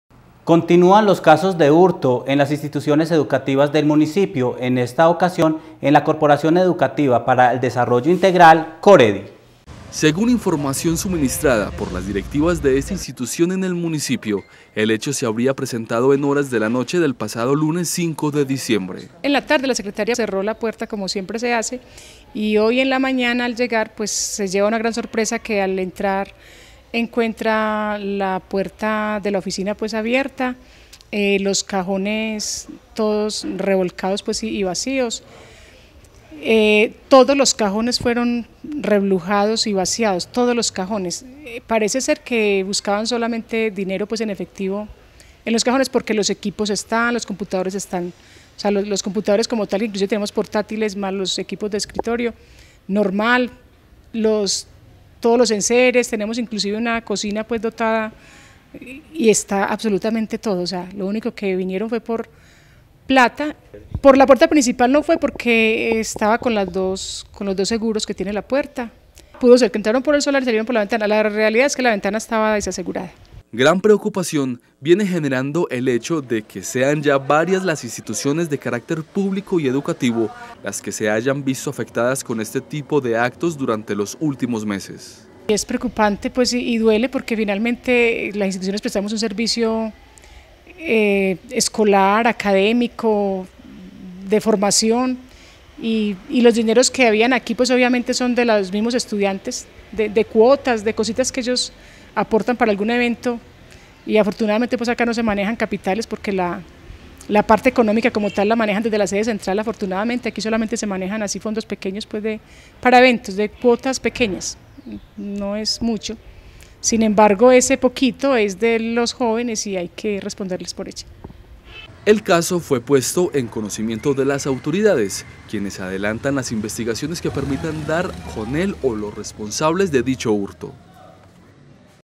COREDI FUE VÍCTIMA DE LOS LADRONES
Continúan los casos de hurto en las instituciones educativas del municipio, en esta ocasión en la Corporación Educativa para el Desarrollo Integral COREDI.